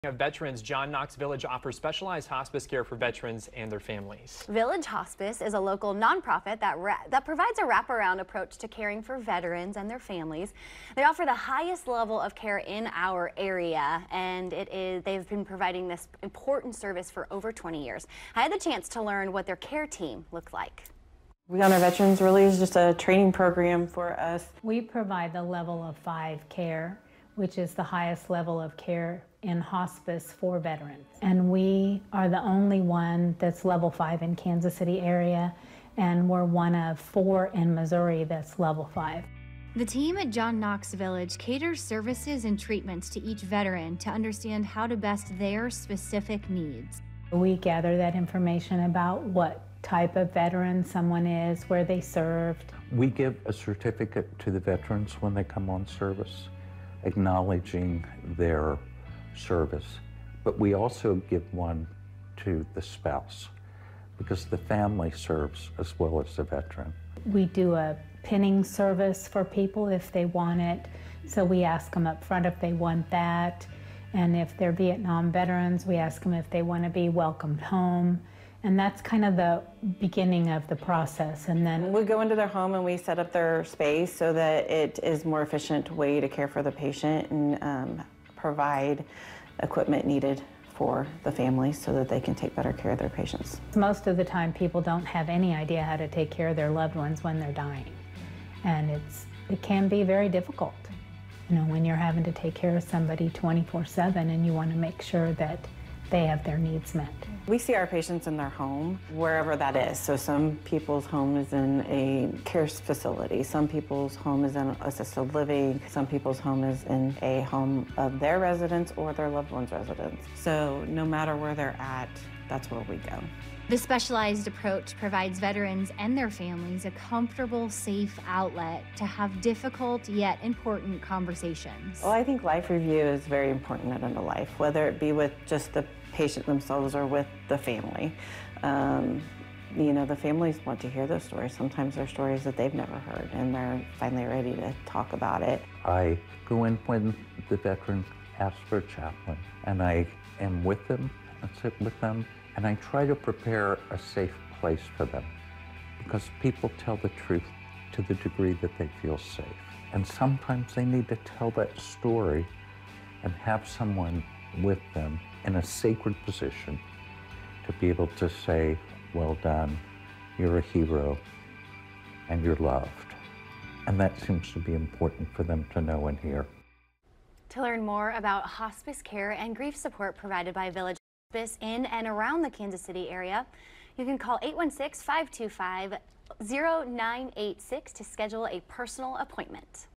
John Knox Village Veterans Hospice Care
At the heart of Village Hospice is a belief that each person deserves to experience more moments of life, and each family deserves to be fully supported and informed. John Knox Village provides the highest level of hospice care for veterans and their families in the Kansas City area. The team is able to customize care for every Veteran and is diligent about meeting their specific needs.